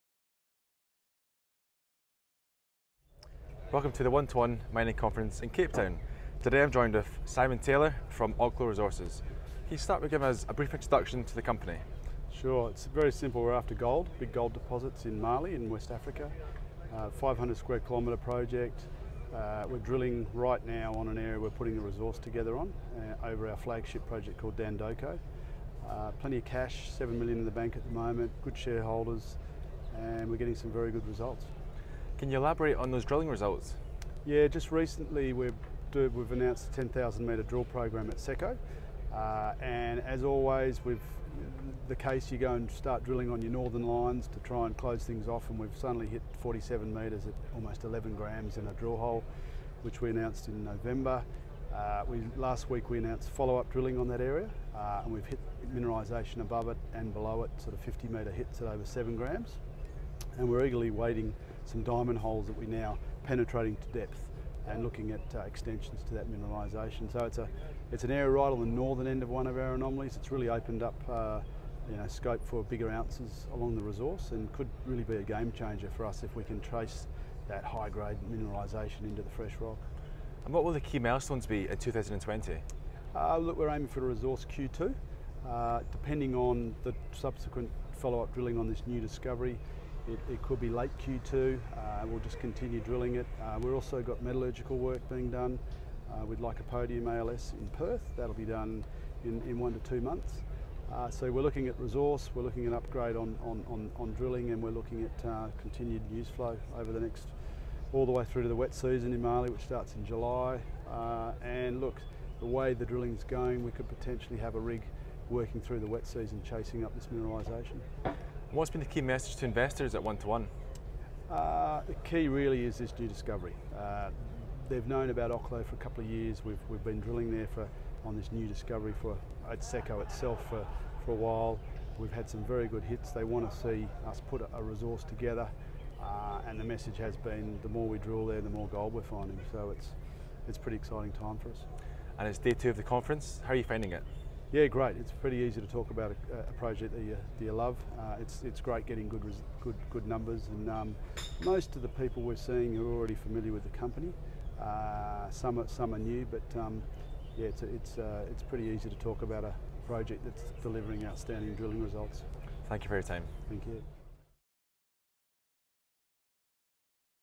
Interview: Simon Taylor, Oklo Resources - 121 Mining Investment Cape Town 2020 Spring
Primary ticker: ASX:OKU
Stage of development: Exploration
Primary minerals: Gold
Project to promote: Dandoko Project – Seko Prospect

Oklo Resources’ focus is its large landholding of 11 gold projects covering 1,400km2 in some of Mali’s most prolific greenstone belts, concentrated in two key areas; West Mali (Dandoko, Kouroufing, Moussala, Sari, Kossaya, Kandiole and Socaf) and South Mali (Yanfolila, Solabougouda, Sirakourou and Kondieba).
Both groups of permits are located in the vicinity of multi-million ounce gold mining operations and recent discoveries. Oklo’s flagship projects of Dandoko and Kouroufing are in West Mali with the Company holding approximately 500km2 in the immediate region.